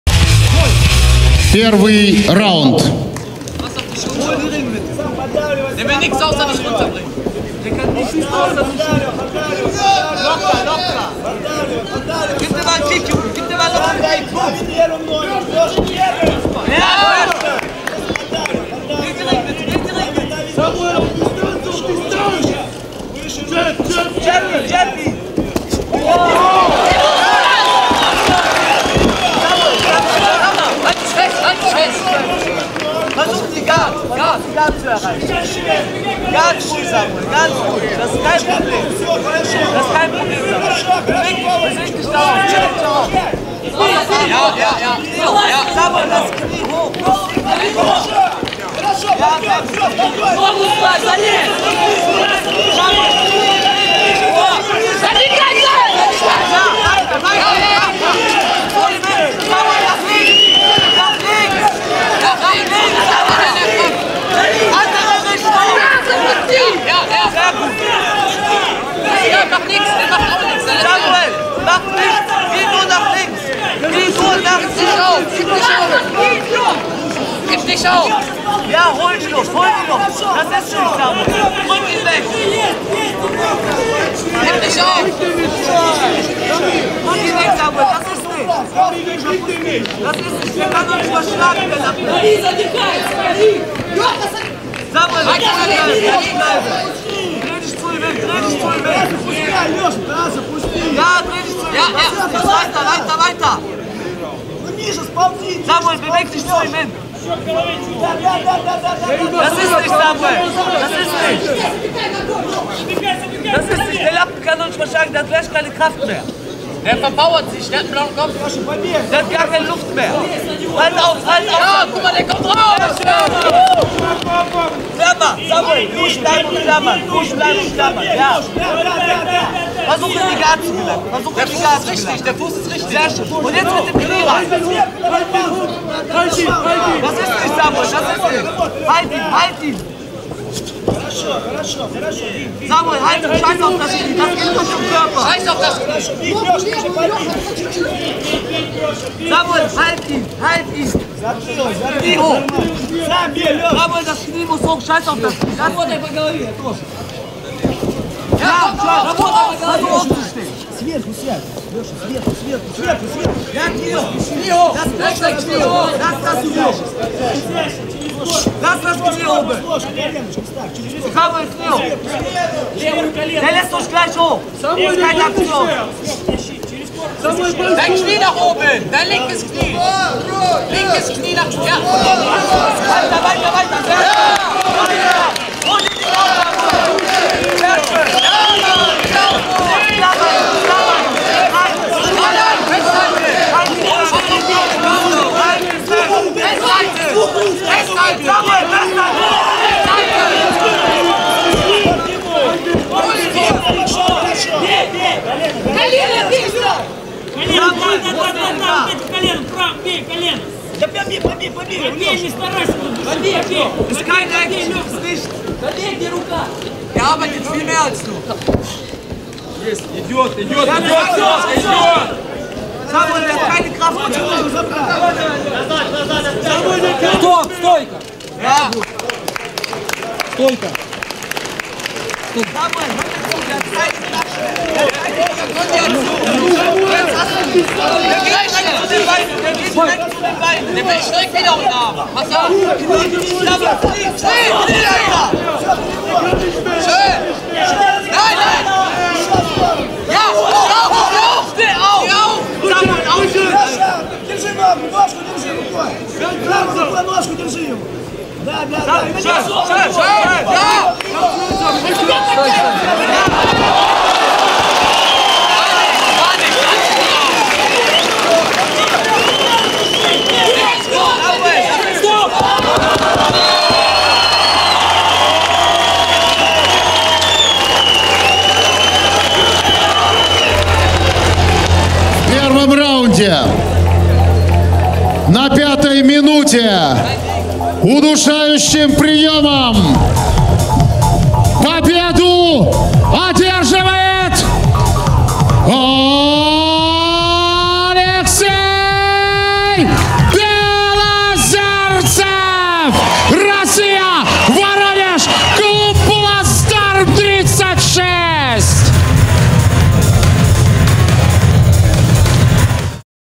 5. Бои без правил. Воронеж. 12.10.2013. БЕЛОЗЕРЦЕВ vs OSUNDU
Белозерцев Алексей - Воронеж, клуб «Плацдарм 36», представляет Союз ММА России
Мастер спорта по вольной борьбе, победитель Всероссийского турнира по смешанным единоборствам «Память спецназа», бронзовый призер Чемпионата России по смешанным единоборствам, чемпион Европы по смешанным единоборствам 2013 года.

Samuel Osundu- Германия, Дюсельдорф, клуб SPORTSCHULE
победитель северного рейна-вестфалии по смешанным единоборствам и победитель северного рейна по К-1.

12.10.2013 -- FIGHT RIOT II -- первый международный турнир по смешанным единоборствам в Воронеже!

ВРОО "Федерация смешанных боевых единоборств MMA", ВРОО "Федерация вольной и греко-римской борьбы", при поддержке Управления по физической культуры и спорта Воронежской области, Администрации городского округа город Воронеж

Место проведения -  Event-Hall Сити-Парк "Град"

Запись и телетрансляция боев - ООО "Продюсерская компания "ВижуМир" (с)